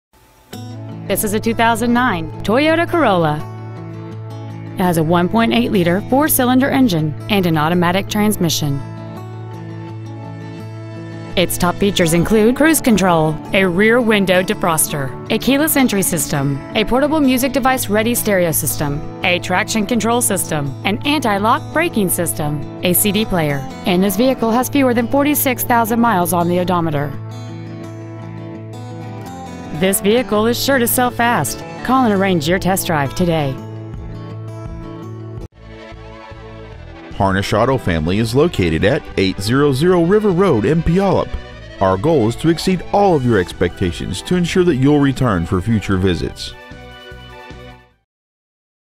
2009 Toyota Corolla Puyallup WA
Year : 2009

 Make : Toyota

 Model : Corolla

 Engine : 1.8L 4CYL

 Trans . : Automatic

 Exterior : Red

 Miles : 45,229

 Interior : Beige

 Stock : R1427

 820 River Road

 2009 Toyota Corolla Puyallup WA